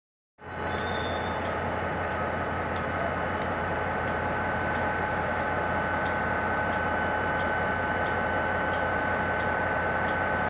CNR CKD 8H 0001 llegando a vía Z de Retiro (para hacerse cargo del Cordobés) y en vía 1 la 9021 (I)
El domingo 23 de julio de 2023 a las